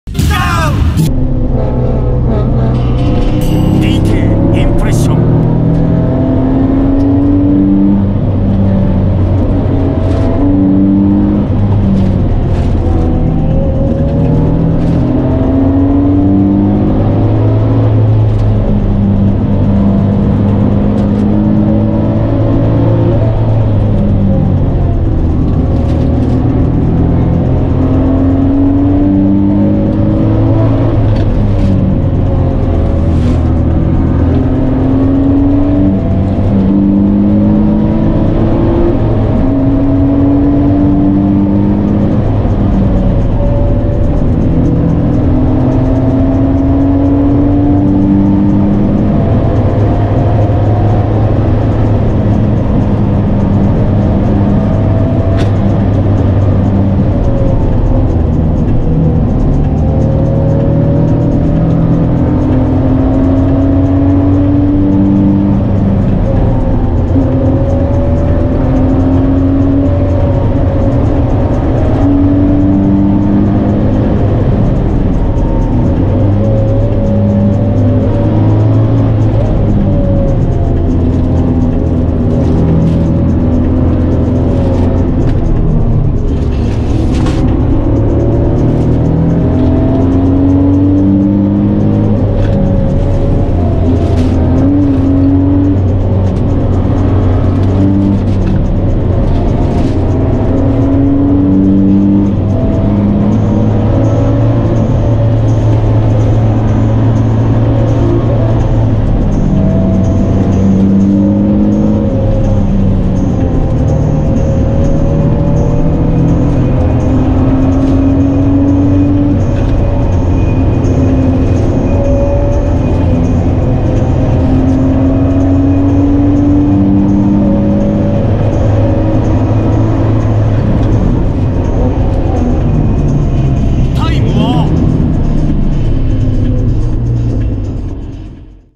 Touge Run | NISMO 350Z & 370Z
350Z NISMO:

300 hp / 360 nm / 1500 kg
Michelin Pilot Sport Cup 2 (245/40 R18 & 265/35 R19)
Kaaz LSD 1.5 Way & 3.69 Final Gear
KW Clubsport

370Z NISMO:

370 hp / 390 nm / 1550 kg
Michelin Pilot Sport Cup 2 (265/35 R19)
NISMO GT L.S.D Pro 1.5 Way & 4.08 Final Gear
KW V3